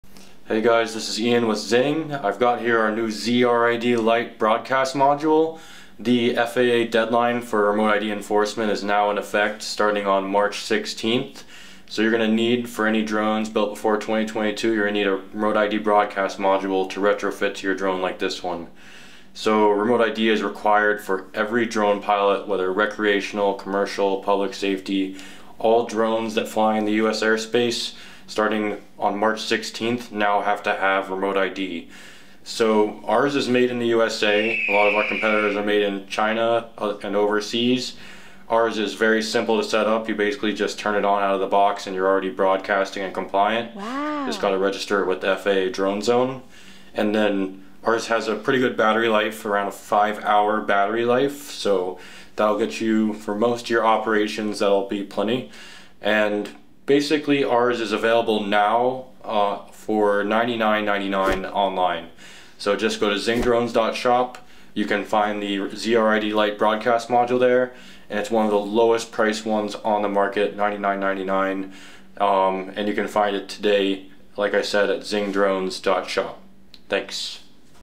Z-RID Lite - FAA Remote ID Compliance (Made In The USA)
FAA Remote ID Enforcement Began on March 16th. The FAA warns drone pilots to Get Compliant or Don't Fly It. Are You Remote ID Ready?